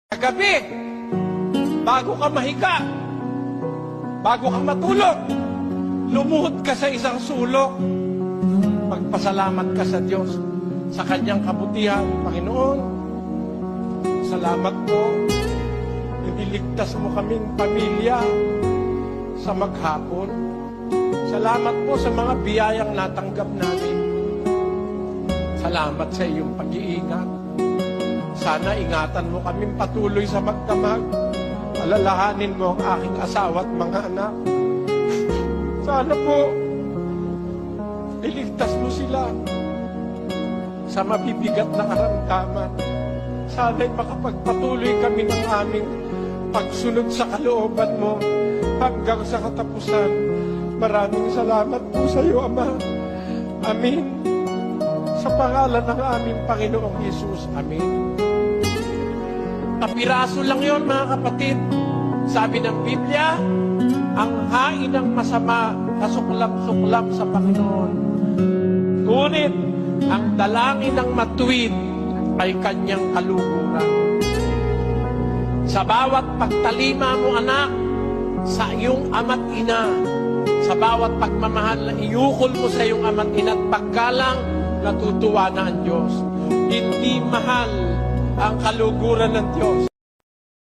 Bago mahiga at matulog magpasalamat sa Dios sa kanyang kabutihan
Aral ni Bro. Eli sa mga kristiyano na kumikilala sa kabutihan ng Ama nasa langit!